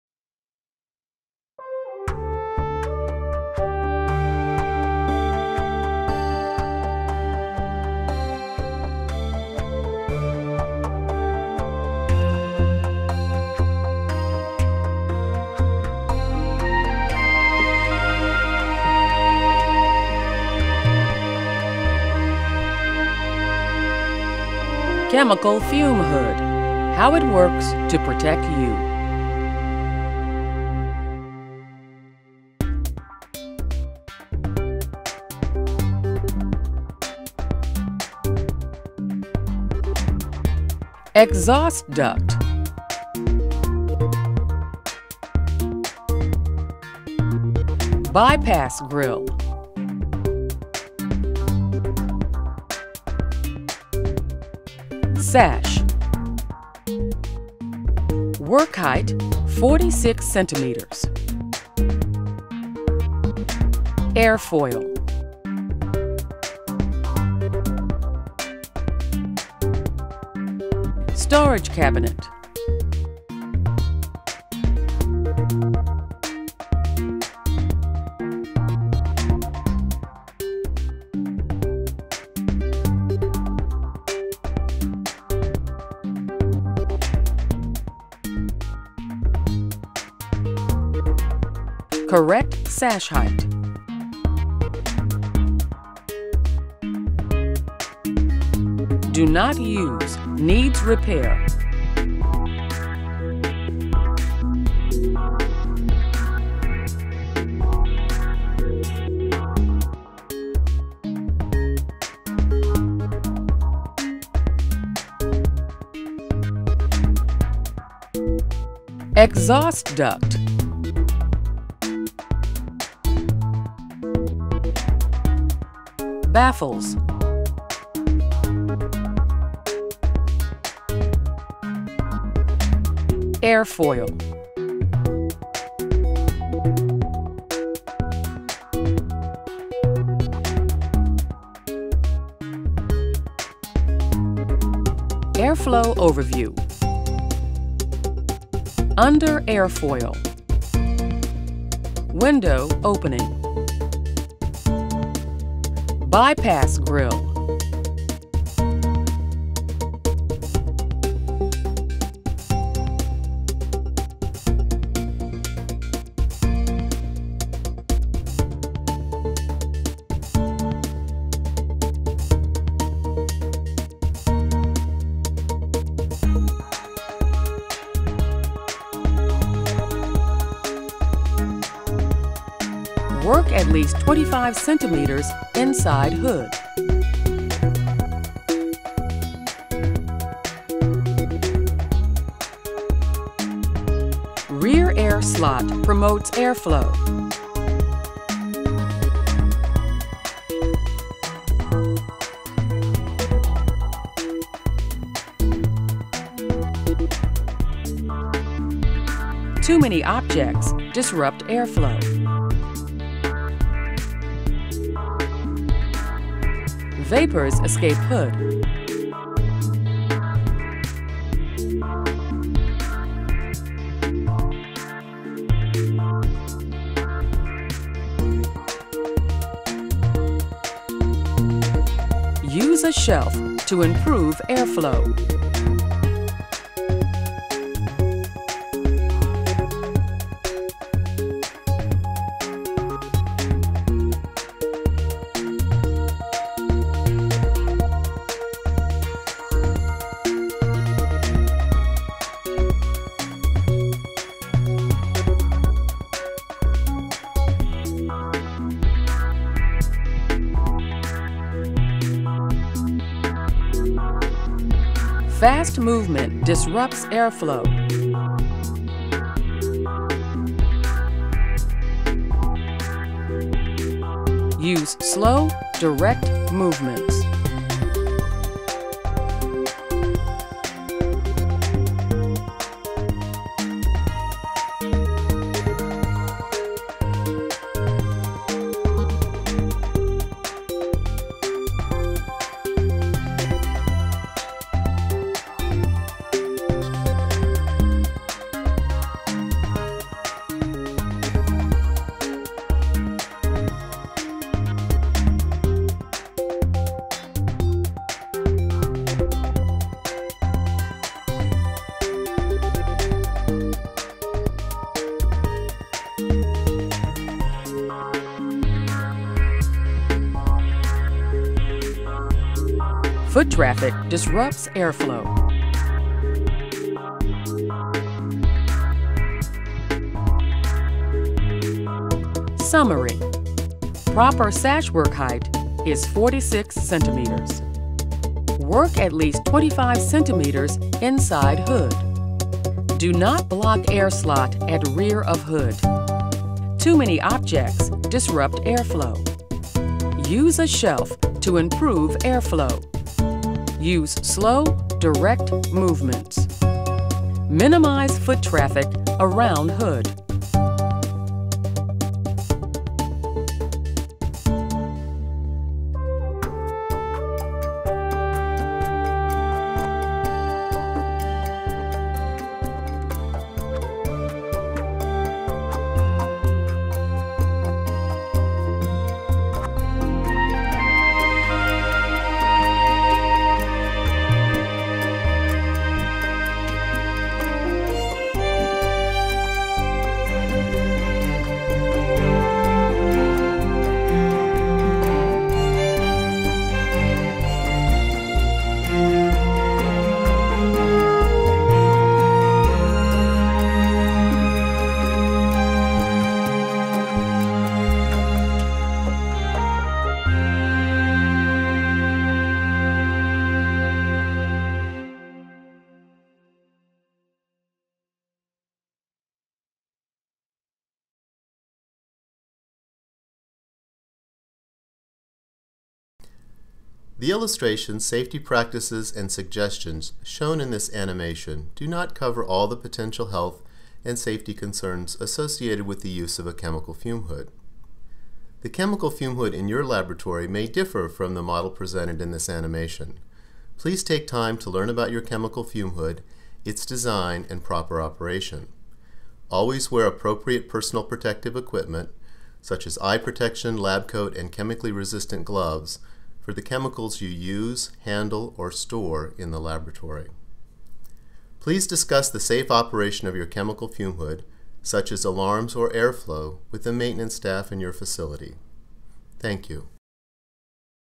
Chemical Fume Hood Animation in Spanish
a laboratory safety project by
Dartmouth College & The National Institute of Health
"Chemical Fume Hood:  How it Works to Protect You"